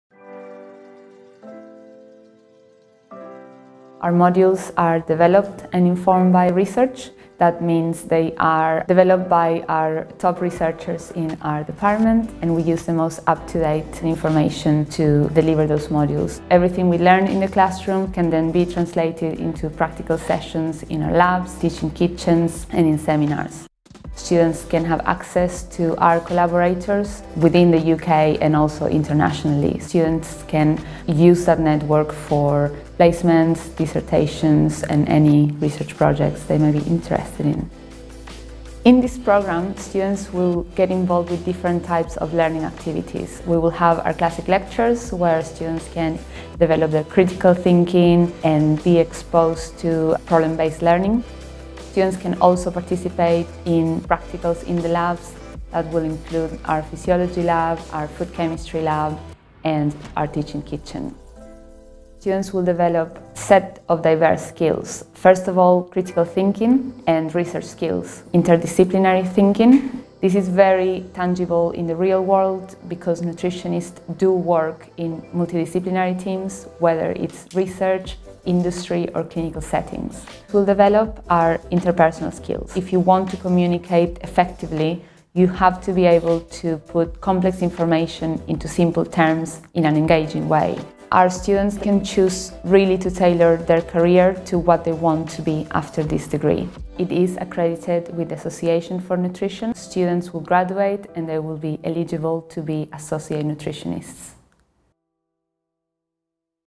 Why study Nutrition at the University of Exeter?
Staff and students from our BSc Nutrition course tell us what makes Nutrition at Exeter unique. More about the course